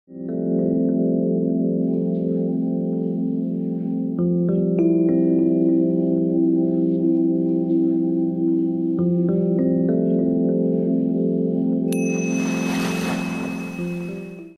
Beach Freely - Moo-ment of Zen
Some people meditate, others meditate beachside, under a tree, wearing a cow mask.